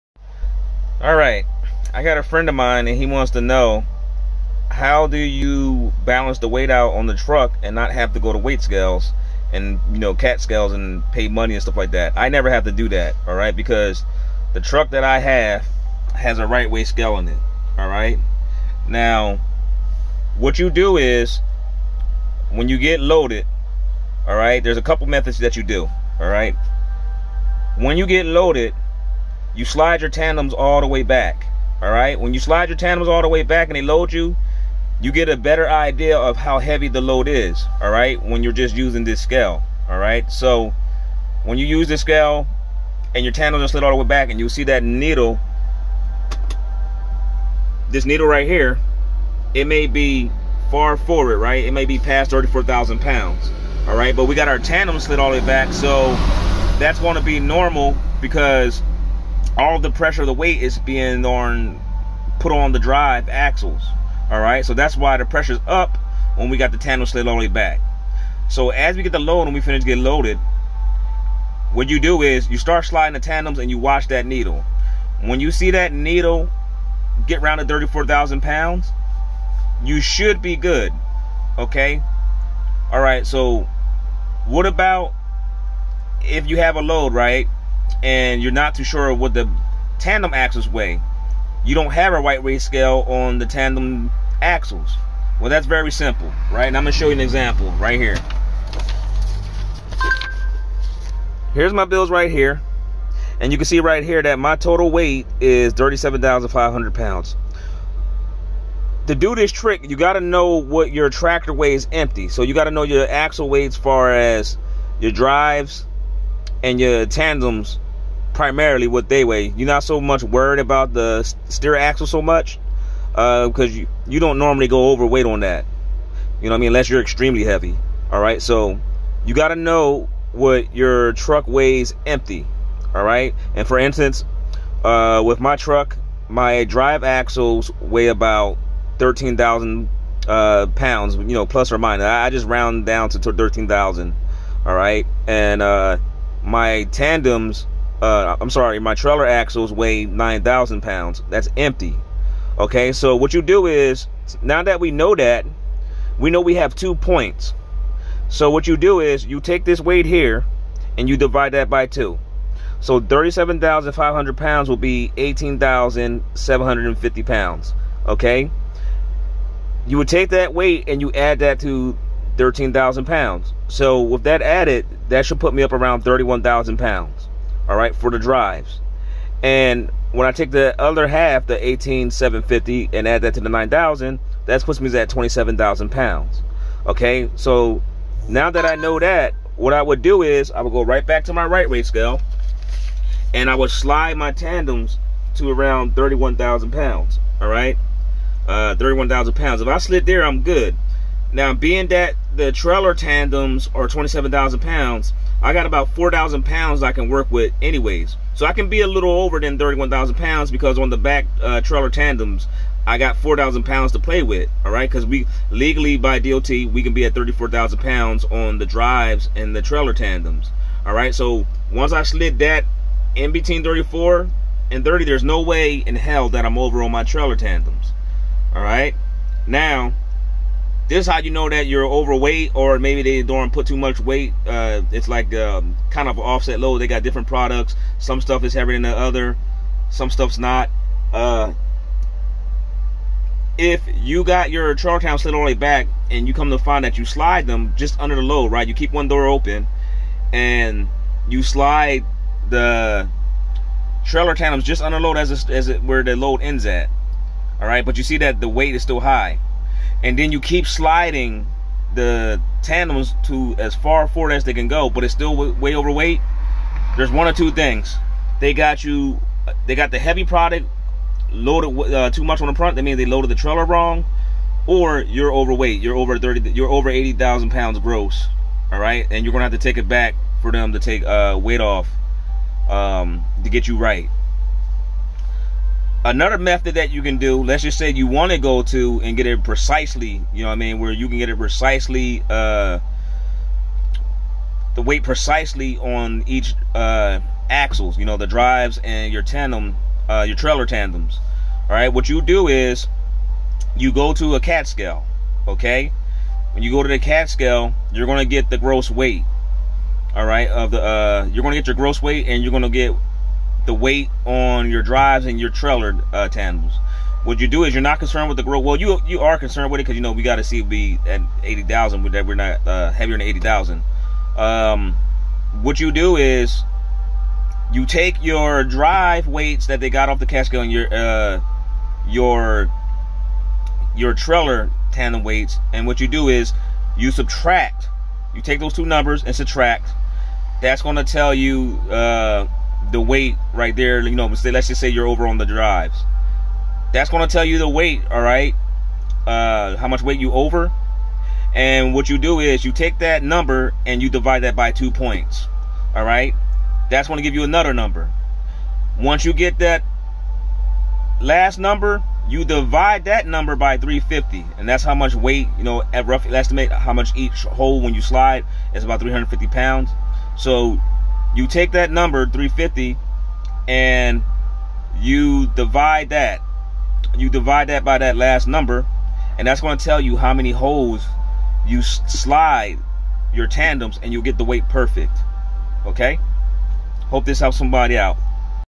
How to adjust weight on axles FREE (D.O.T legal 3 methods)
How to get the weight on axles DOT legal 3 methods. Adjust your weight on axles for free, dot legal methods to adjust weigh on axles.